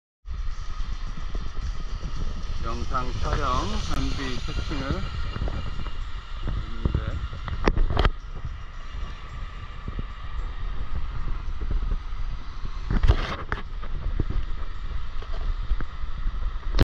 나코야트 Nacoyacht 거치대로 생방송 하기
렌즈 쪽에 구멍을 뚫어야 제대로 보임, 좌우가 바뀜